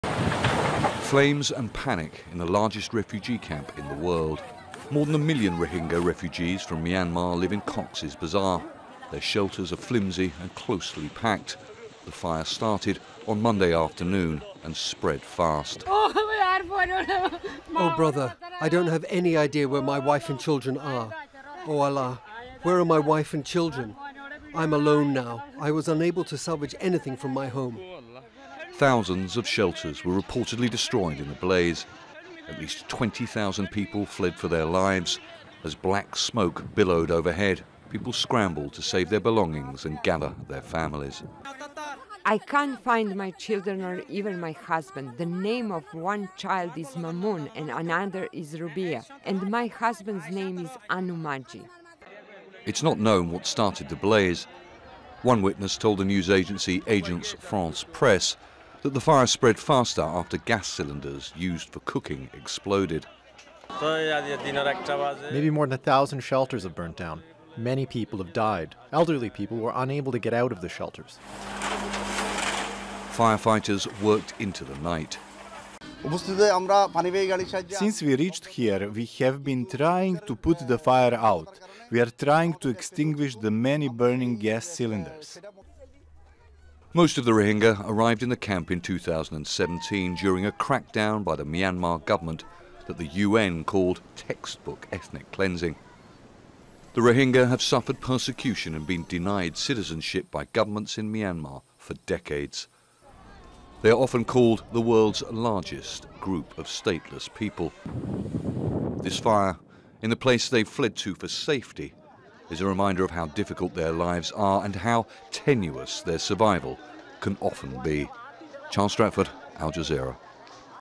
Bangladesh probes deadly fire at Rohingya camp, 400 missing
At least 15 people have been killed and thousands left homeless after a major fire at a Rohingya refugee camp in Bangladesh.
It swept through the Balukhali camp in Cox's Bazar.
Al Jazeera's Charles Stratford reports.